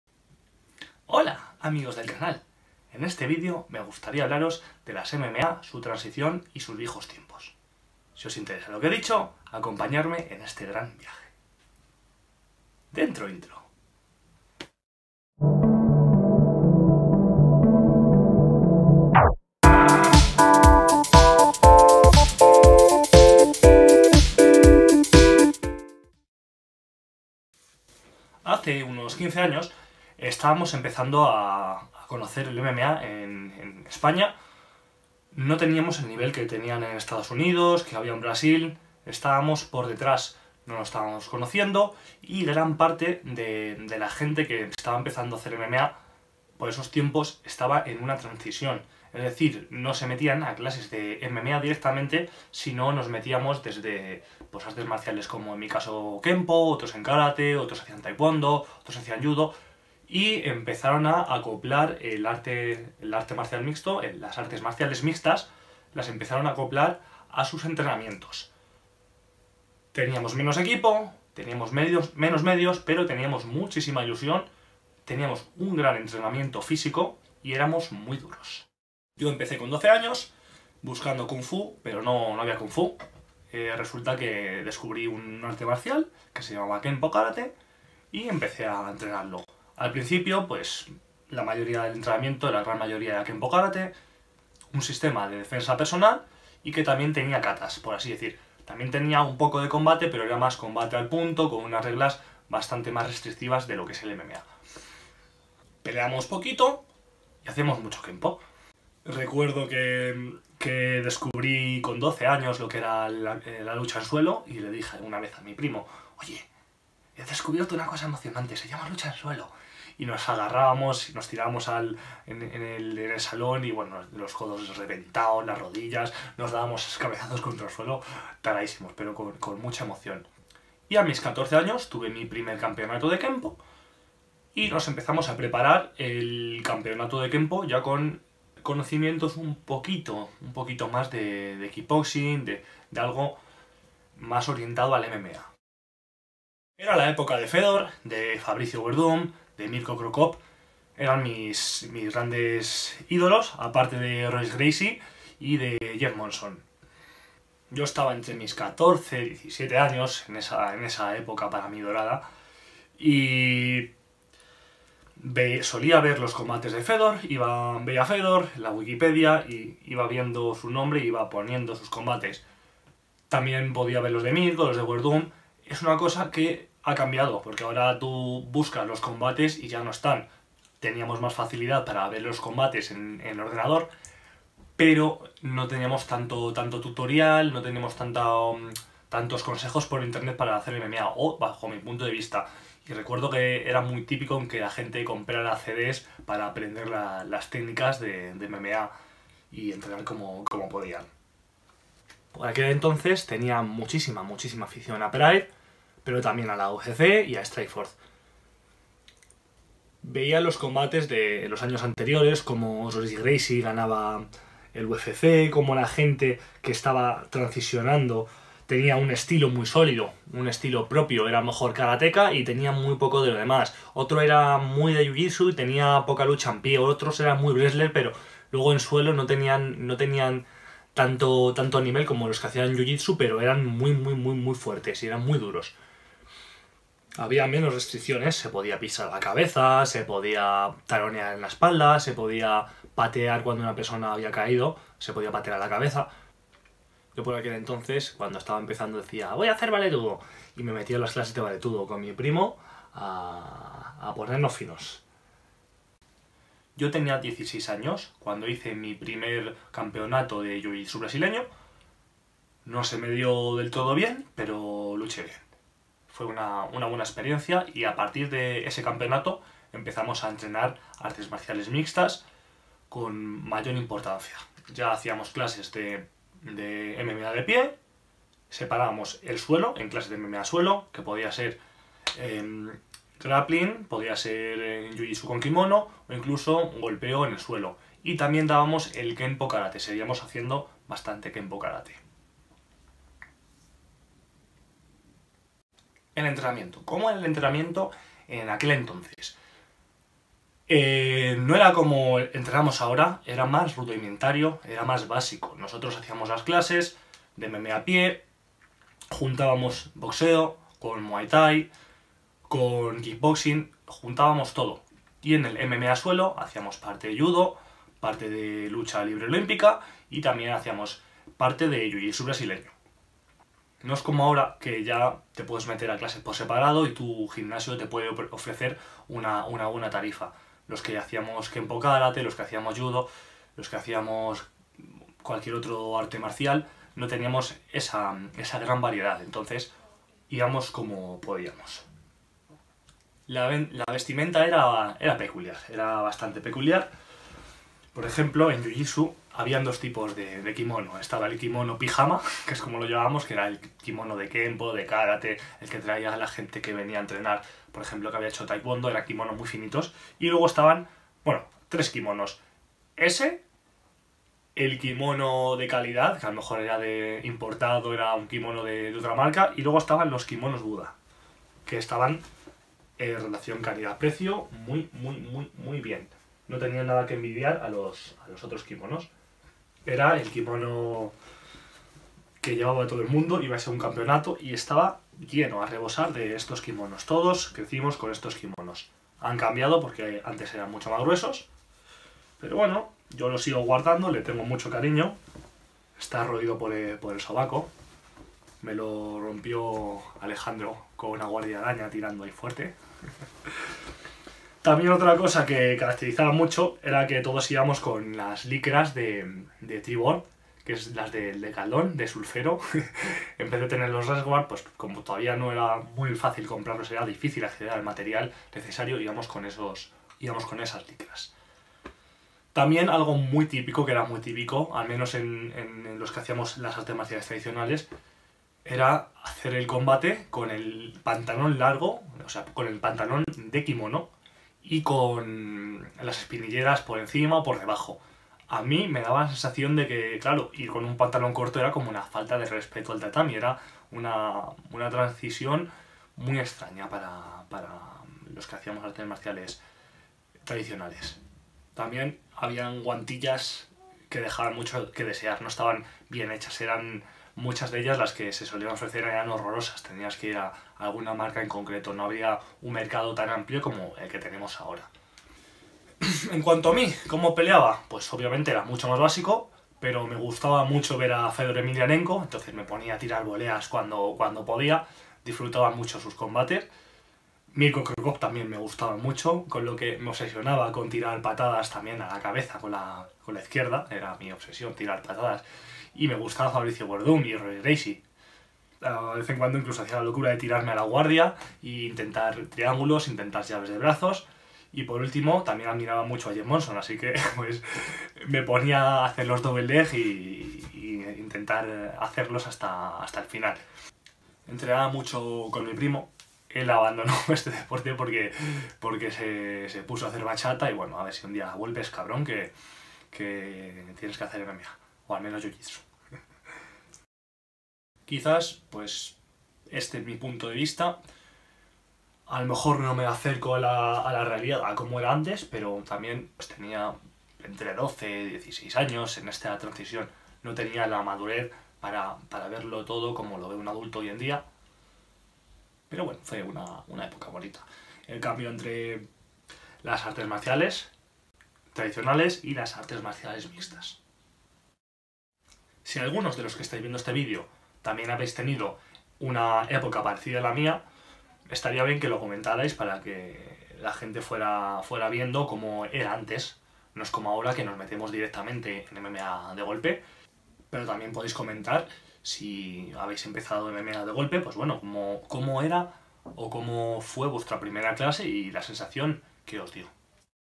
La Transición Del MMA en España: Mi Experiencia
Hola de nuevo, os veo en otro video mas, esta vez hablo de los "viejos tiempos" en el MMA, deporte que conozco desde los 12 años.
En especial  cuento como la gente practicaba Artes Marciales Tradicionles, hacian su esfuerzo por ir adaptándose al nuevo movimiento que estaba presente para así poder progresar su camino artistas marciales/luchadores, desde mi punto de vista, un adolescente con mucha emoción por este deporte de combate.

Como siempre, mil gracias por vuestra visita, y si os a gustado, os agradecería aún mas vuestro apoyo con un like, suscripcion, campanita etc etc etc...  :)